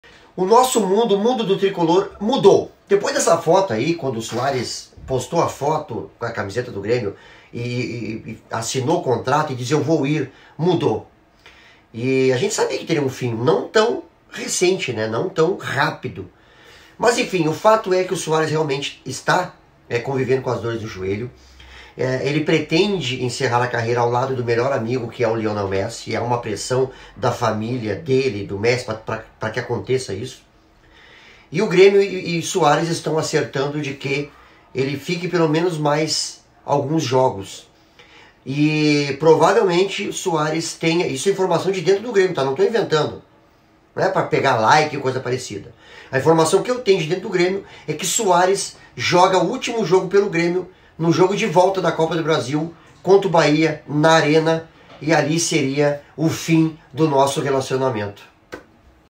BOMBA suarez e a despedida do gremio 🥲
bomba Suárez fará despedida após jogo de volta pela Copa do Brasil contra o Bahia, dia 12/07 na Arena.